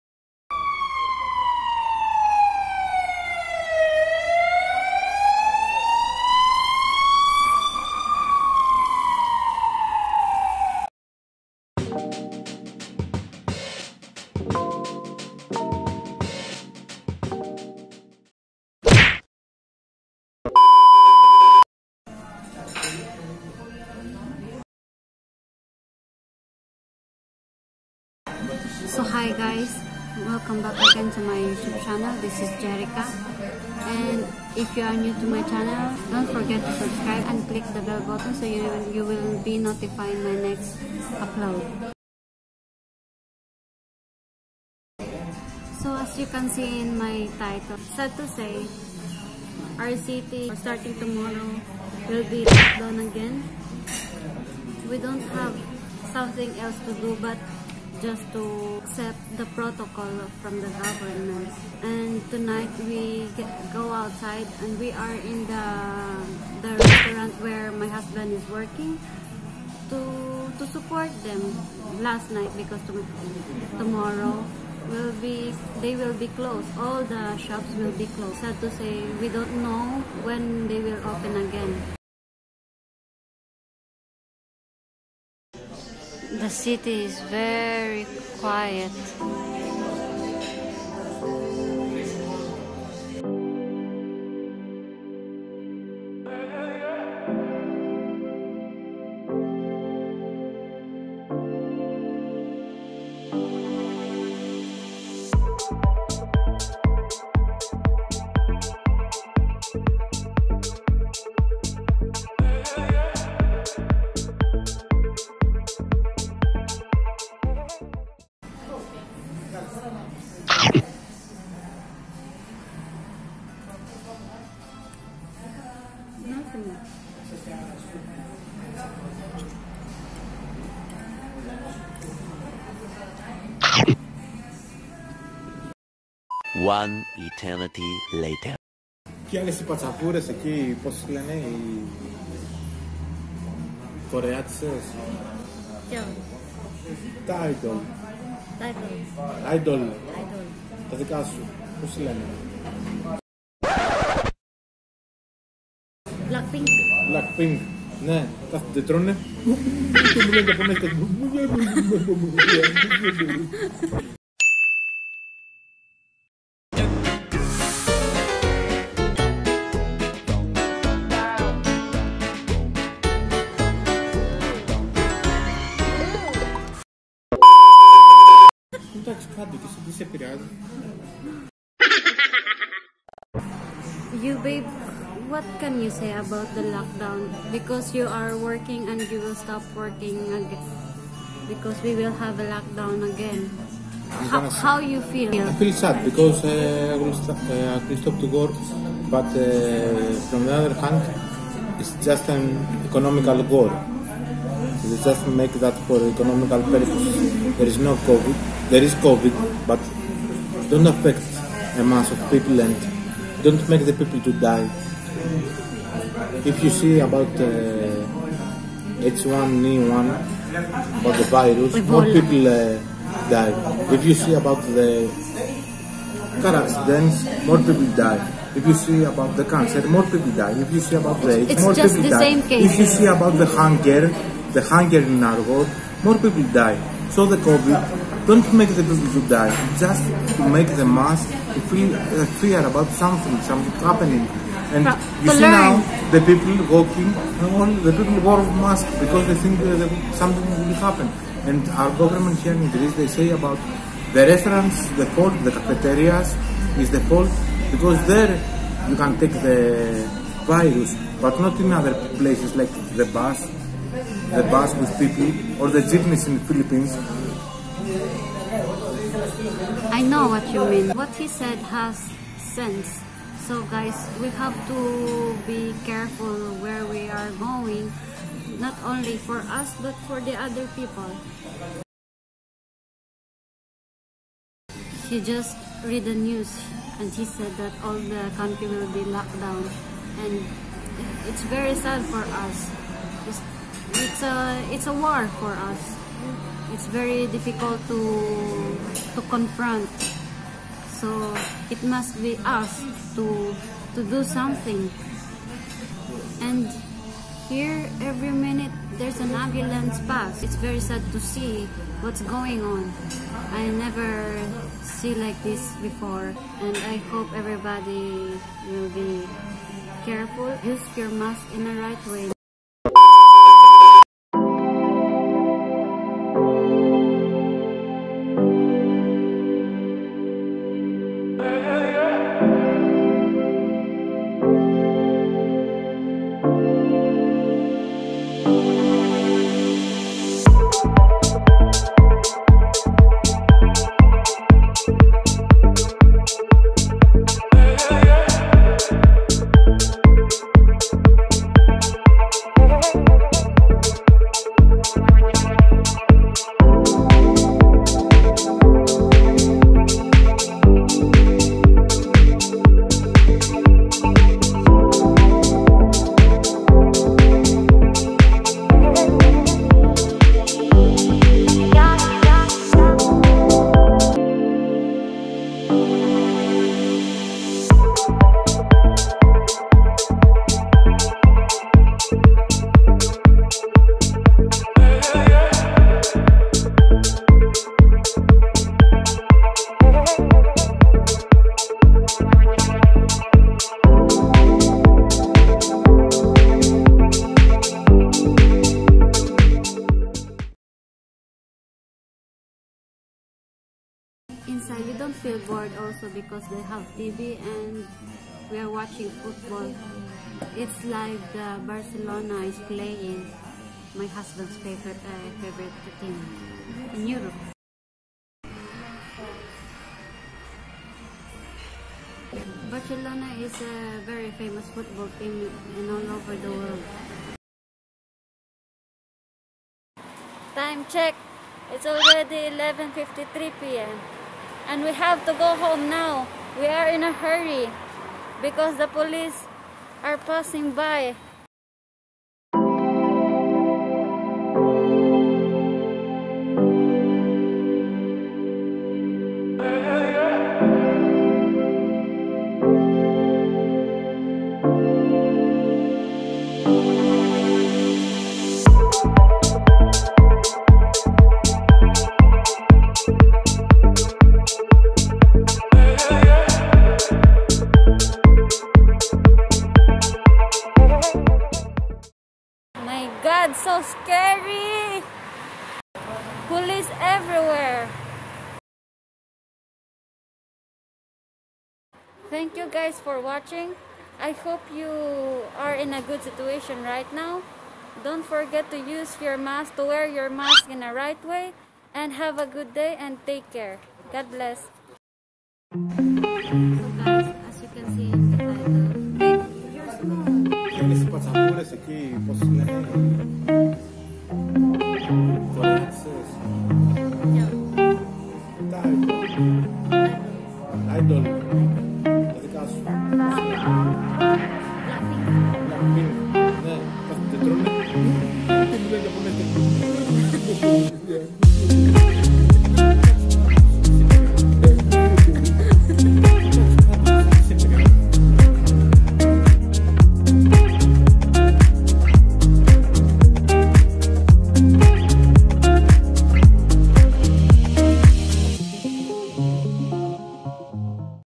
WE'RE IN LOCKDOWN AGAIN!!
Click HD for a better quality.

Prime Minister Kyriakos Mitsotakis announced the new measures the government will take in order to stop the spread of Covid-19. In the High Risk Zone, the operation of all dining areas is suspended, except for home deliveries and receiving a receipt from the store. In short, the Prime Minister announced the following to combat COVID-19.

Use of a mask everywhere, indoors and outdoors.

Daily curfews from midnight to 5:00 in the morning.

Complete distance learning in universities.

No gatherings.

*Eleftheriou Benizelou 7, Anatoli Ioannina
Greece 45221

Willing to join in my membership program?